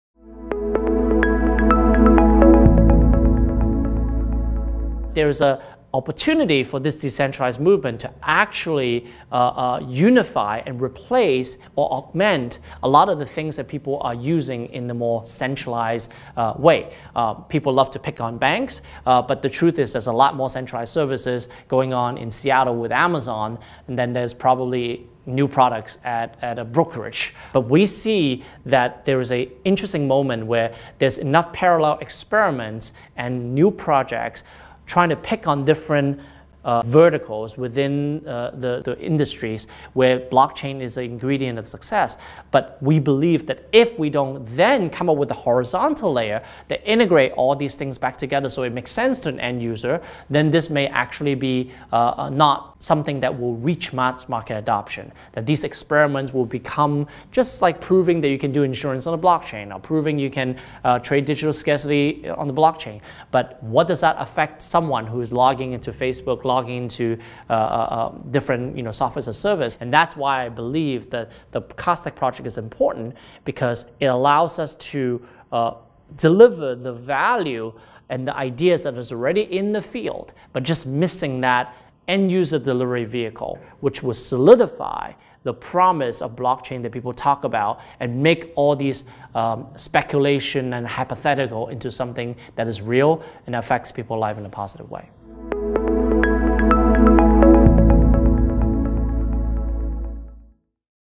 The Blockchain World Needs a Unifying Layer — Cardstack Founder Chris Tse
If we want to decentralize the Internet, we must build a unifying experience layer for blockchain.

Be a part of the Cardstack community!